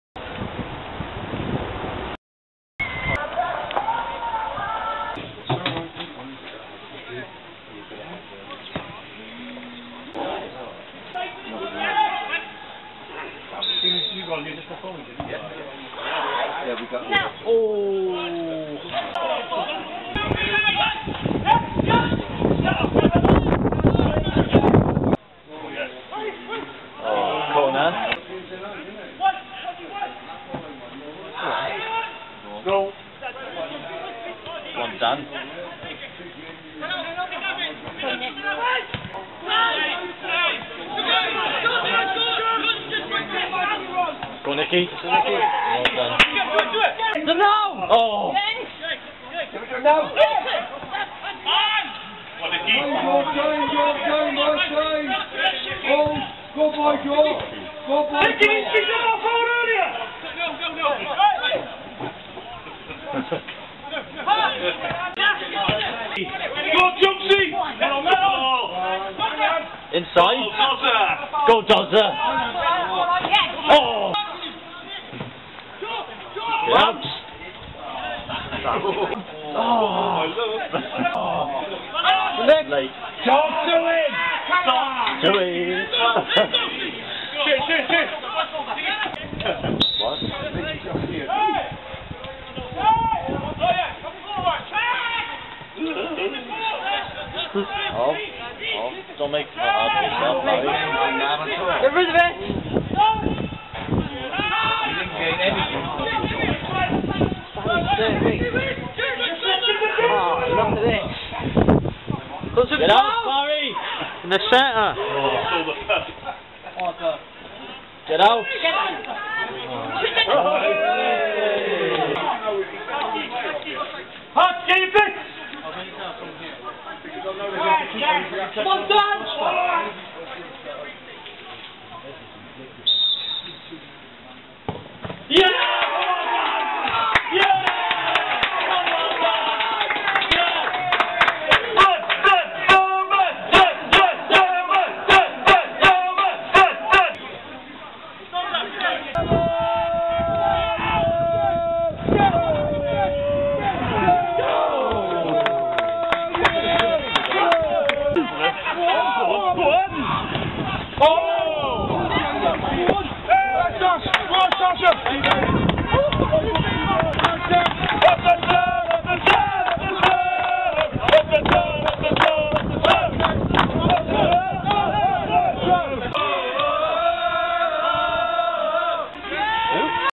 Goytre United 2 Barry Town 3
Action from an action-packed match at Glenhafod Park, as Barry ran out 3-2 winners against Goytre United.

Footage includes a penalty save from Barry goalkeeper Dan Bradley. This MacWhirter Welsh League match was played on Saturday 27th April, 2013.

Watch out for some brief bonus Welsh Premier footage at the end, as a handful of Barry supporters made the short journey to nearby Victoria Road to catch the second half of Port Talbot Town vs Airbus.